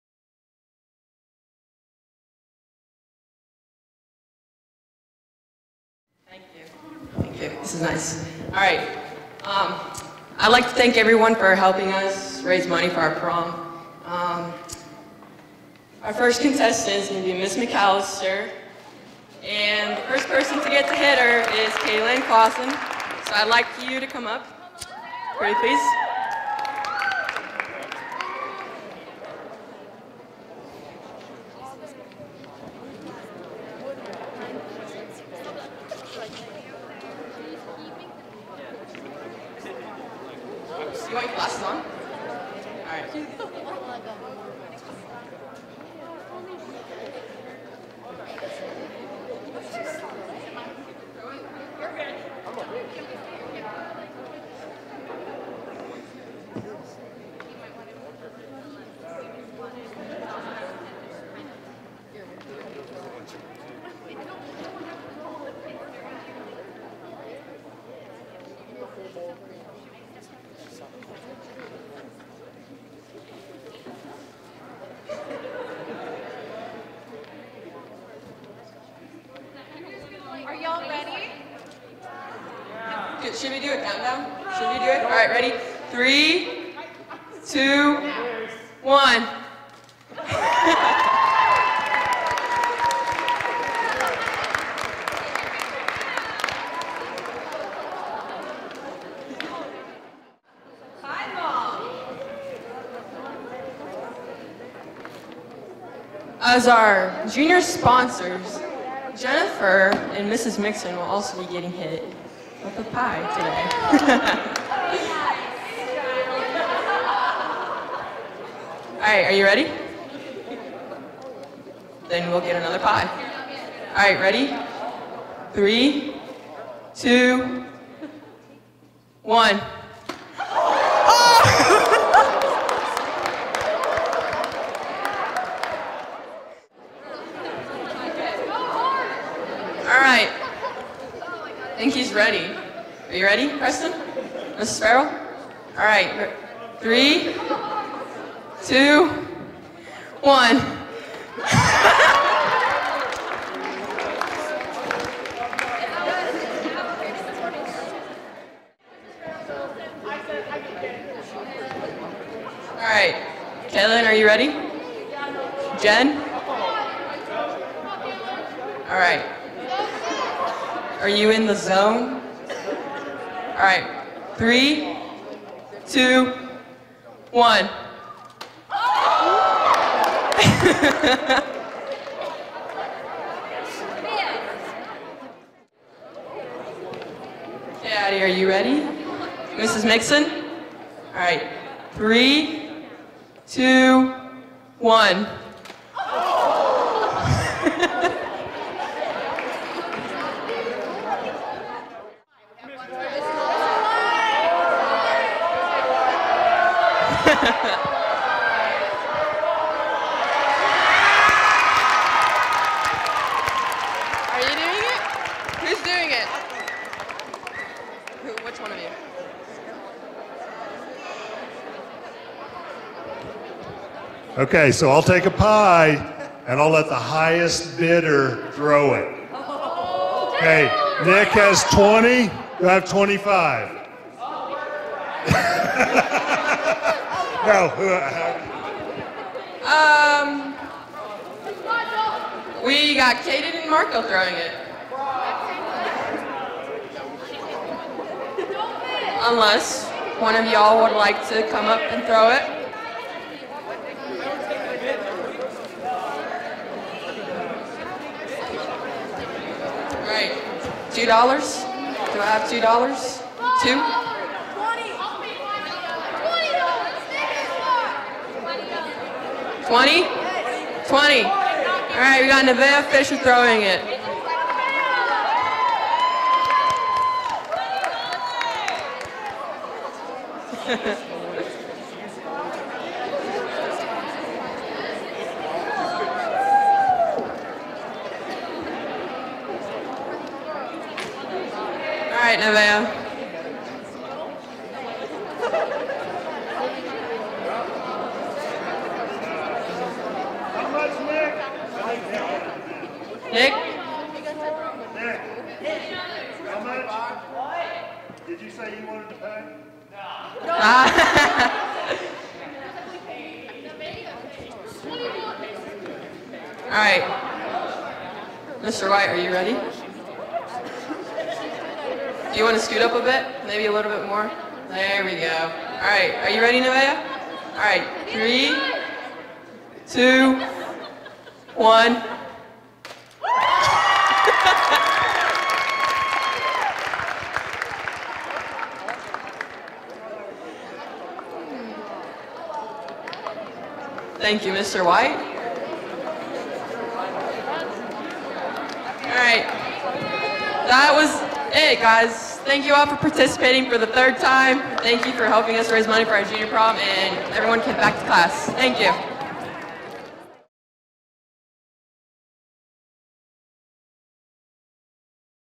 Thanksgiving Pies
TV3 Anchor Intro:  Yesterday, the Junior Class was making “Thanksgiving Pies,” but it used no pumpkin.